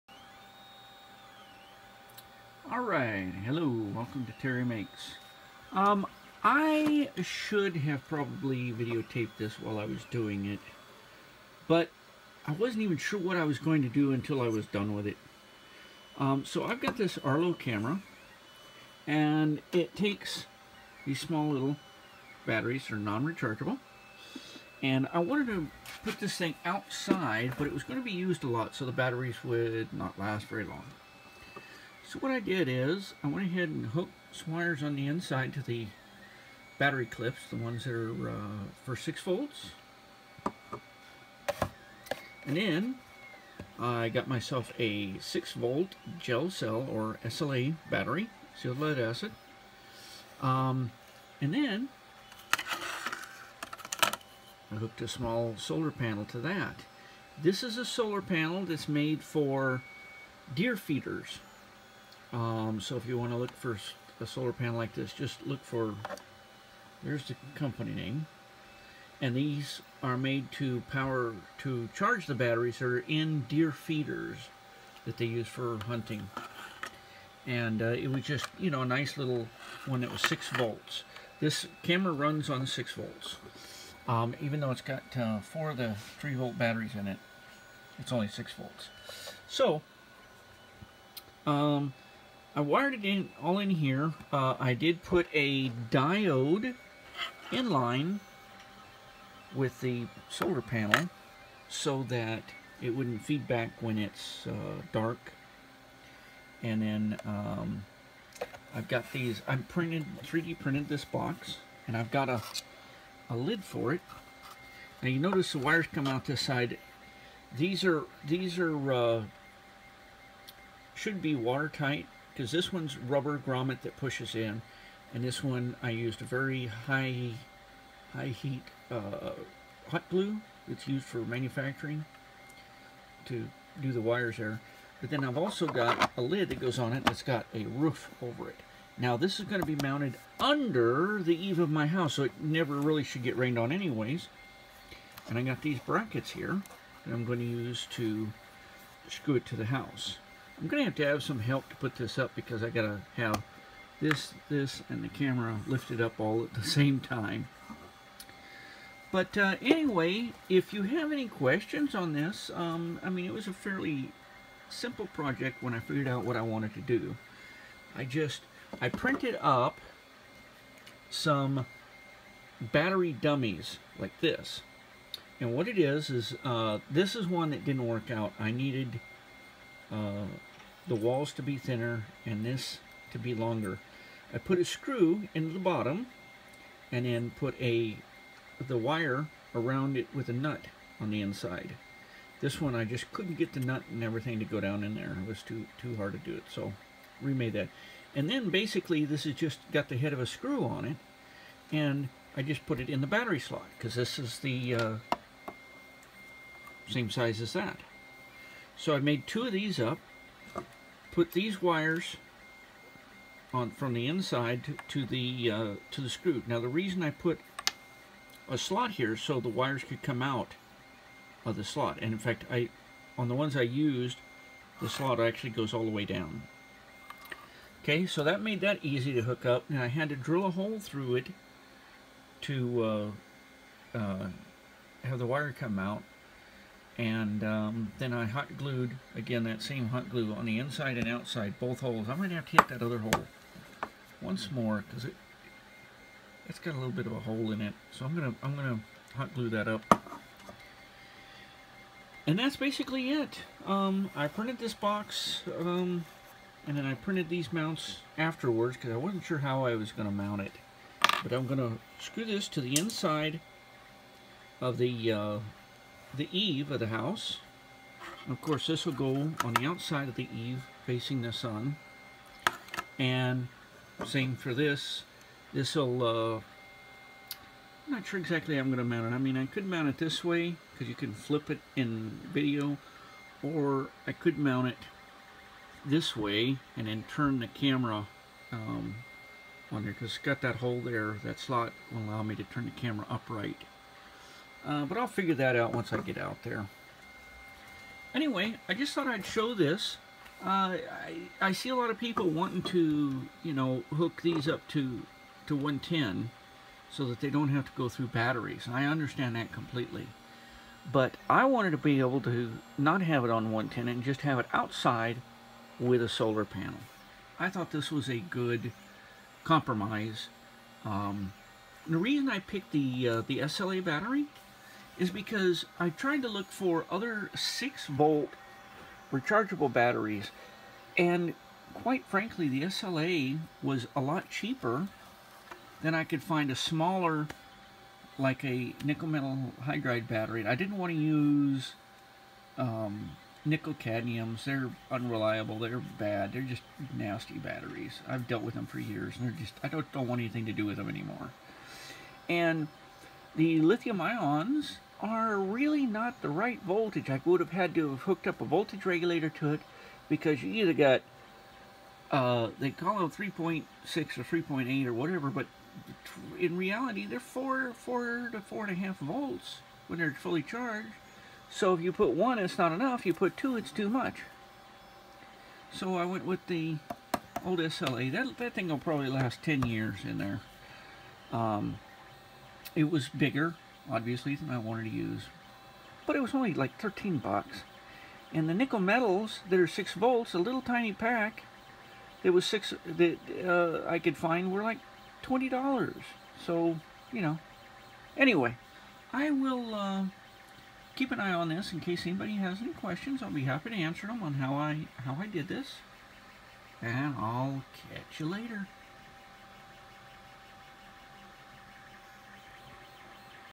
Arlo camera solar pier hack
I wanted to run my Arlo security camera outdoor full-time. I didn't want to have to go and change the battery all the time, so I hacked it with a large battery and solar panel